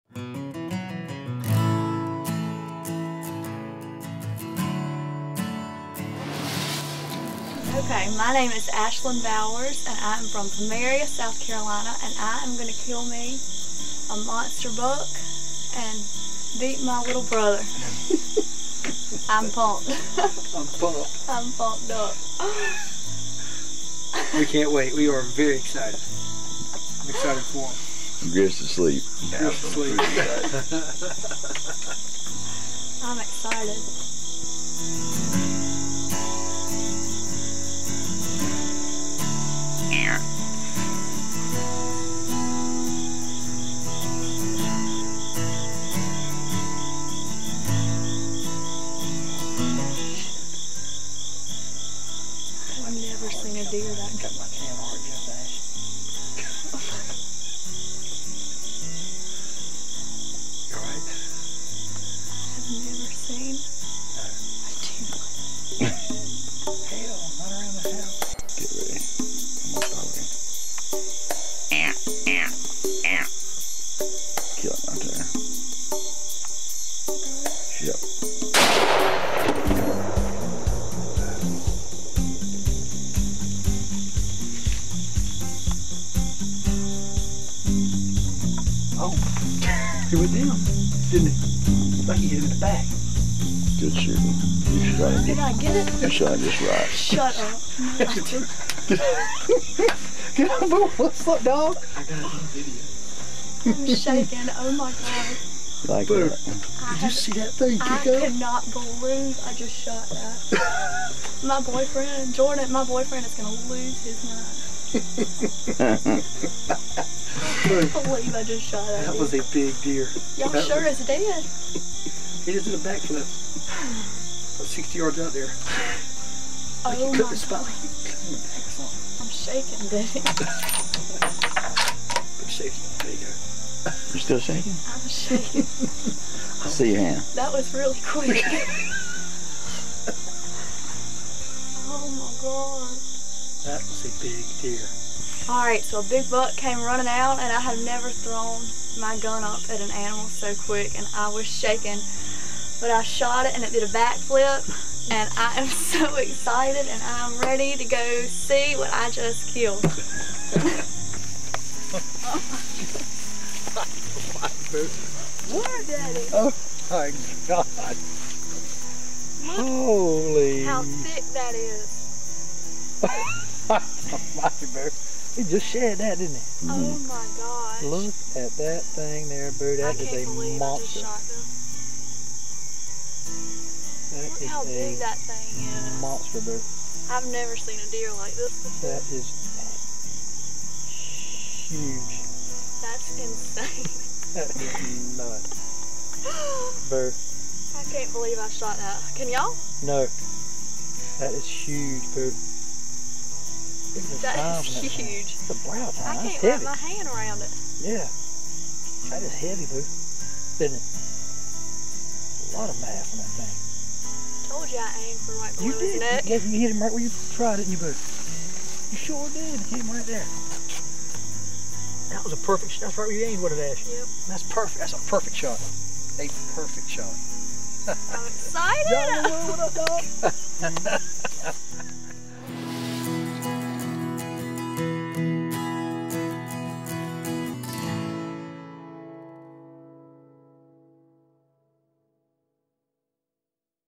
Ashlins Trophy Buck
Ashlin has the experience of a lifetime!  Join us as we dig into the Samsons Mountain archives and relive this special moment!